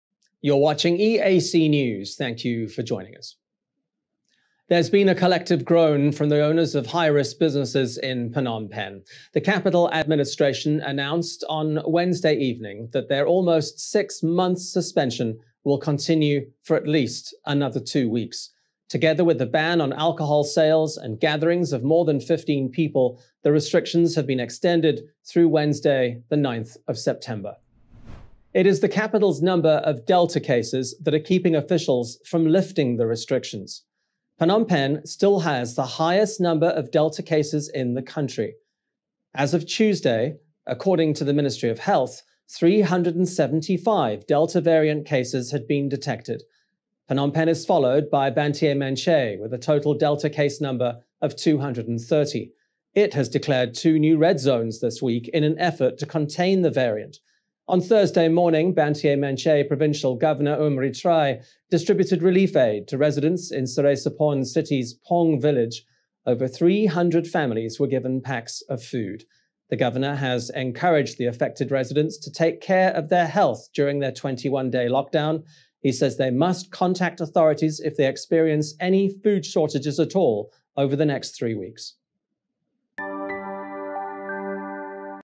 Collective Groan from High Risk Businesses in Phnom Penh
Phnom Penh: There’s been a collective groan from the owners of high-risk businesses in Phnom Penh. The Capital Administration announced on Wednesday evening that their almost six-month suspension will continue for at least another two weeks. Together with the ban on alcohol sales, and gatherings of more than 15 people, the restrictions have been extended through Wednesday 9 September.

It’s the capital’s number of Delta cases that are keeping officials from lifting the restrictions. Phnom Penh still has the highest number of Delta cases in the country. As of Tuesday, according to the Ministry of Health, 375 Delta variant cases had been detected. Phnom Penh is followed by Banteay Meanchey with a total Delta case number of 230.

It has declared two new red zones this week in an effort to contain the variant. On Thursday morning, Banteay Meanchey Provincial Governor Oum Reatrey distributed relief aid to residents in Serey Sophorn City’s Pong Village. Over 300 families were given packs of food. The governor has encouraged the affected residents to take care of their health during lockdown. He says they must contact authorities if they experience any food shortages over the next three weeks.